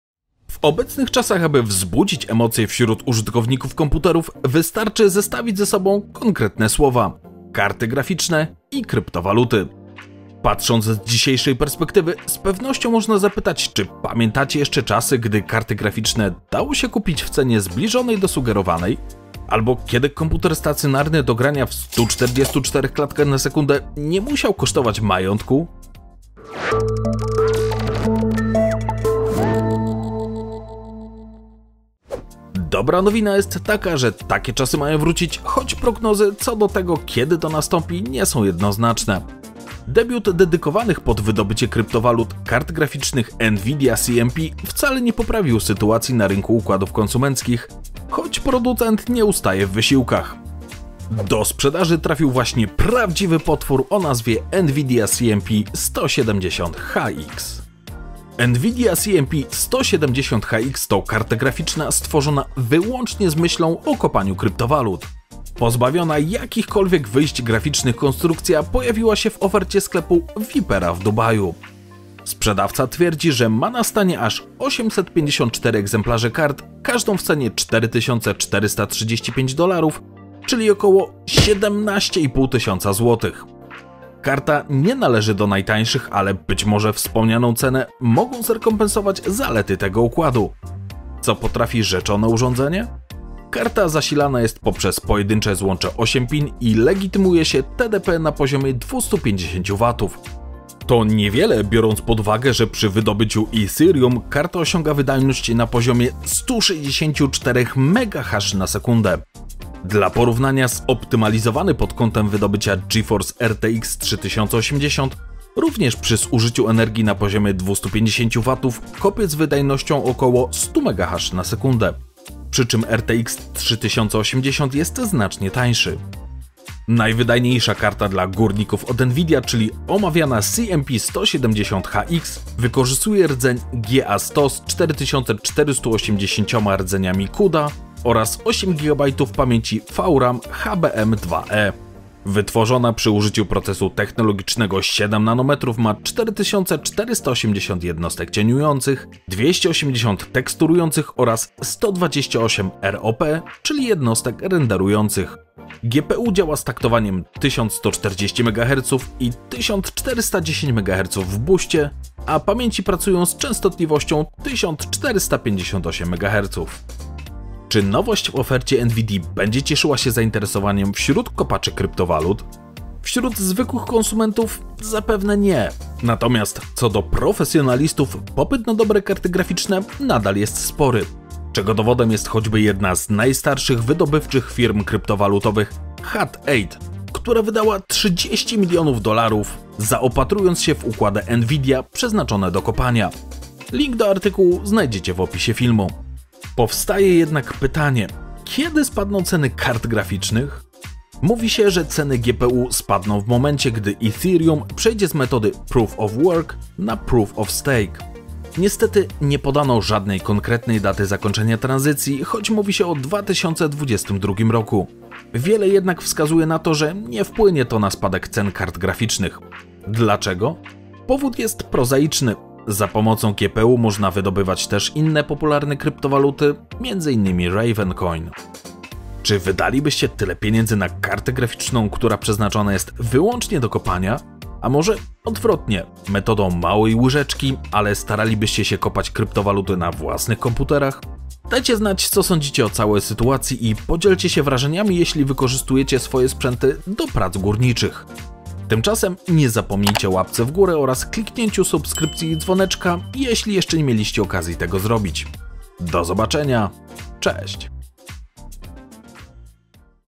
Najlepsza karta do kopania kryptowalut od NVIDIA
Zobacz również: Firma górników kupiła grafiki Nvidia CMP HX za aż 30 milionów dolarów